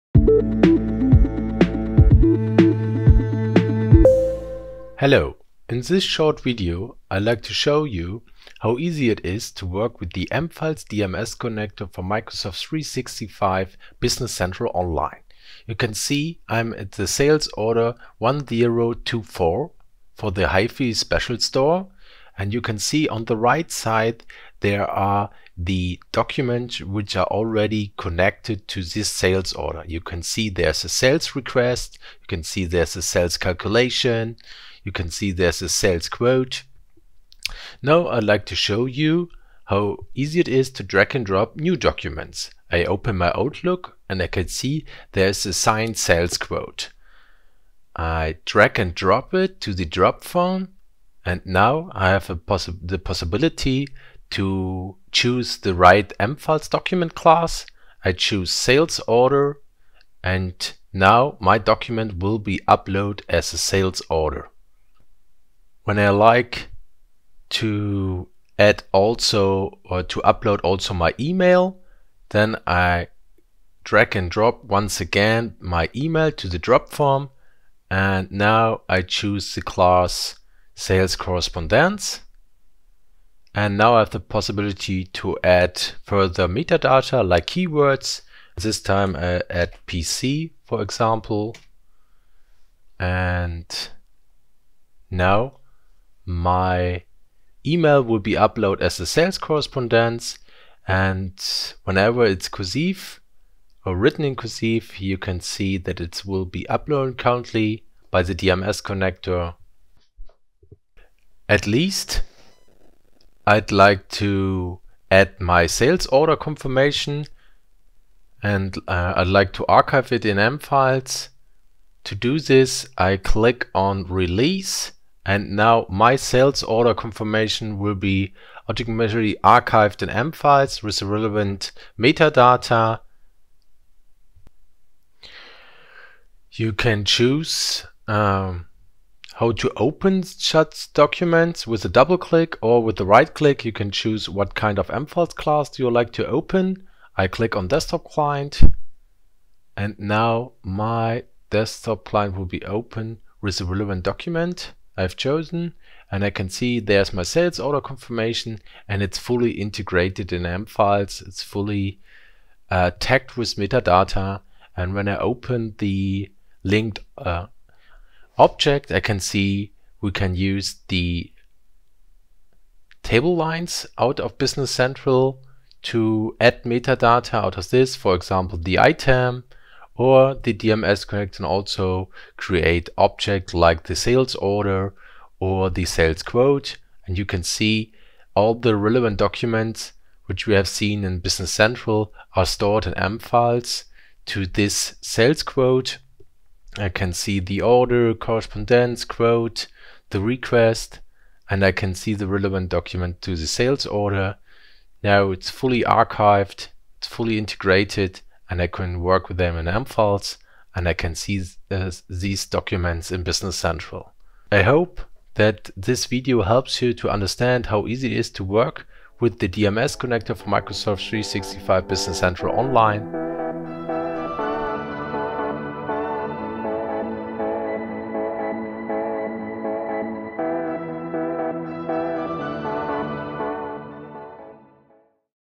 M-Files DMS Connector for Microsoft Dynamics 365 Business Central Online
With the M-Files interface DMS Connector, documents such as invoices, quotations, delivery notes and other electronic documents such as e-mails can be easily transferred to M-Files with master data and processes from Dynamics 365 Business Central / NAV.

How does the DMS Connector work?
When master data and transactions are transferred, all documents from the ERP system are automatically provided with metadata from M-Files. In this way, users can view scanned documents from daily incoming mail in Business Central / NAV and assign them to the correct transactions without having to leave their familiar Business Central environment.

In this video we will explain the M-Files DMS Connector and show you step by step how it can be used to connect the ECM system M-Files to the ERP solution Microsoft Business Central Online.

0:00 Introduction
0:37 Document upload via drag & drop
1:50 PDF archiving of BC Reports using the example of sales order confirmation
2:28 Object upload of BC records as M-Files objects